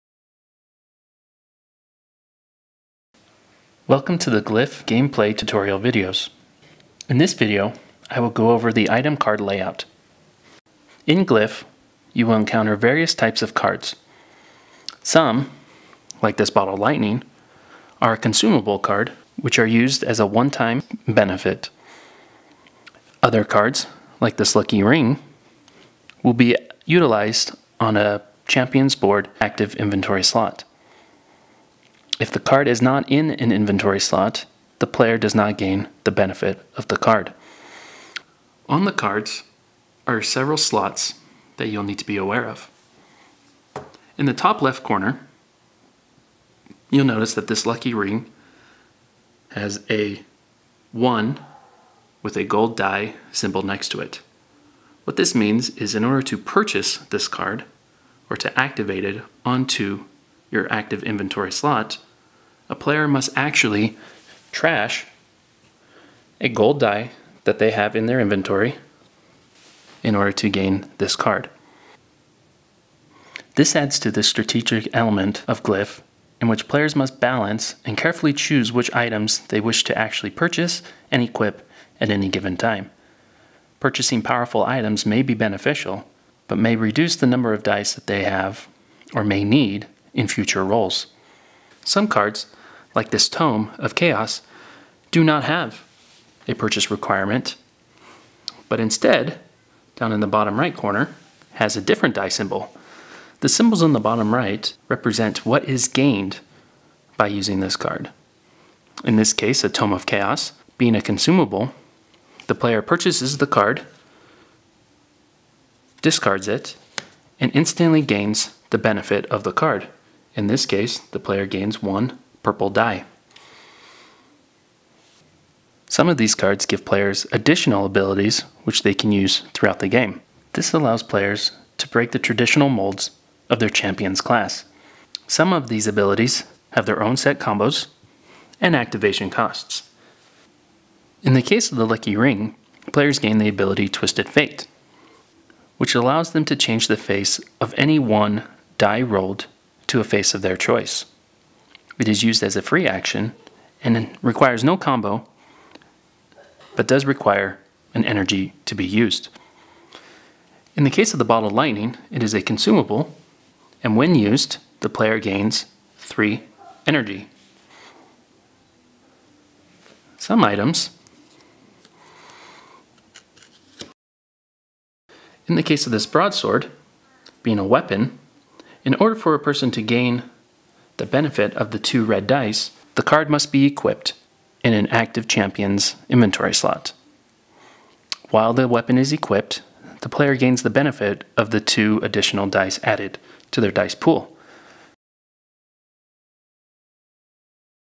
Glyph Gameplay Video - Item Cards
This video highlights the item cards and the role they play in Glyph. Please note these are prototype components and not representative of the final product.

Glyph will be on Kickstarter from November 1 - December 6, 2016

Designed by Justin Potter, Glyph is a dice-building, strategic combat game for two players, set in the fictional land Tundrus; where two tribes have vied for dominance over the region for generations. Your task is to form a team of 3 champions and defeat your opponent via the power of mysterious glyphs.